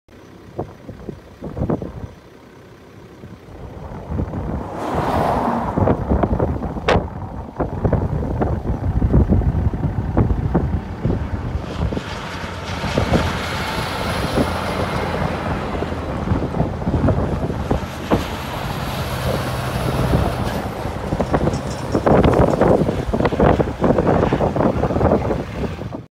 TOLL x2 Kenworth roadtrains
2 Toll Kenworth roadtrains head north up the Pimba Jump Ups..